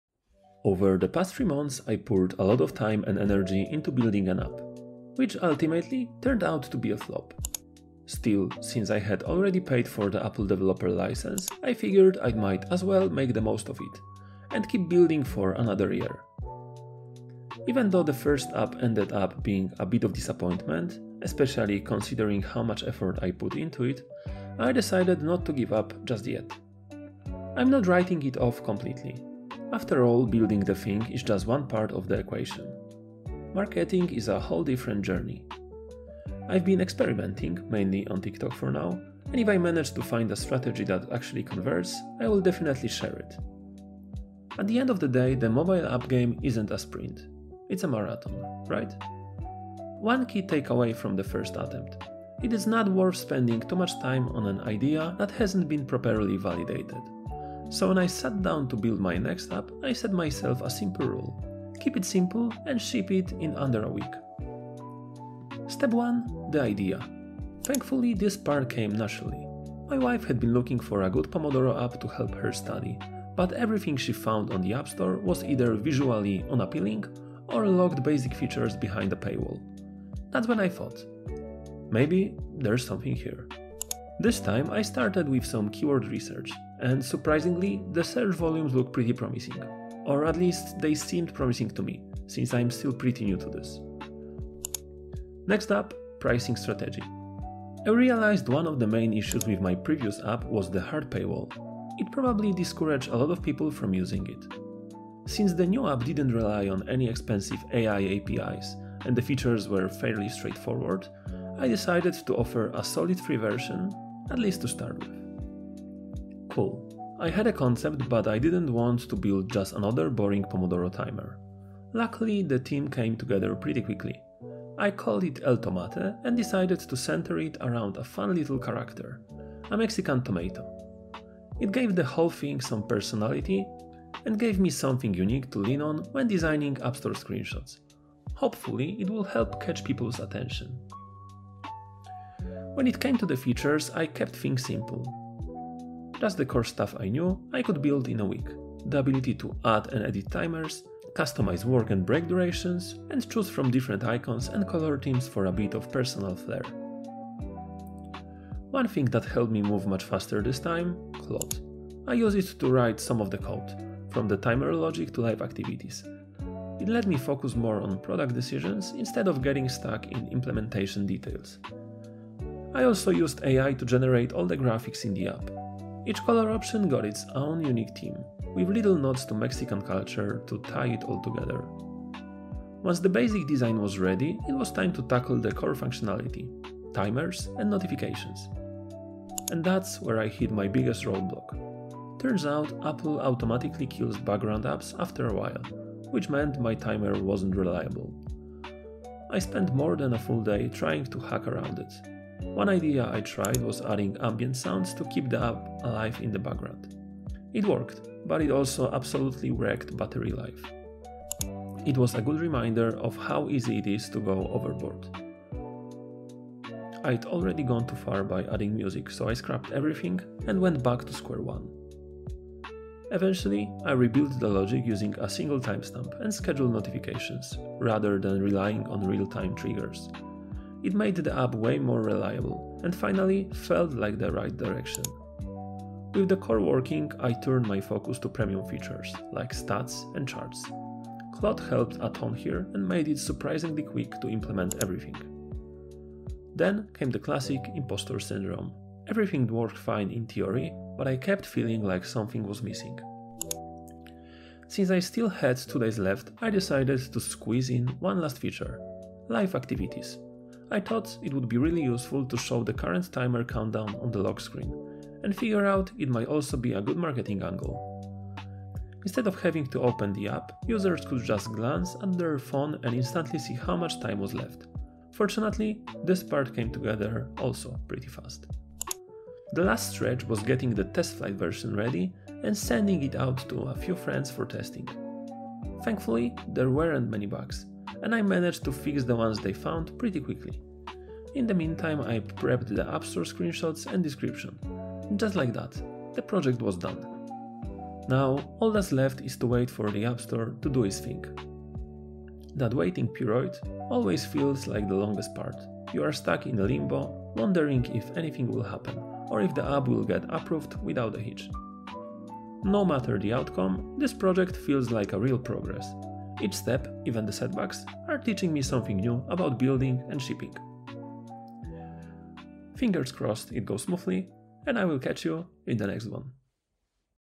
How I Built and Shipped Another App in One Week (step by step / with AI)
I built a Pomodoro app in just one week - after my first app totally flopped. In this video, I walk you through the full story: what went wrong, what I learned, and how I approached this new build differently.

From choosing the idea to launching on the App Store, this one was a wild (and surprisingly fun) sprint.

00:00 - The flop that started it all
00:51 - A better approach the second time
01:57 - Designing El Tomate
02:40 - Development
04:40 - Submitted to the App Store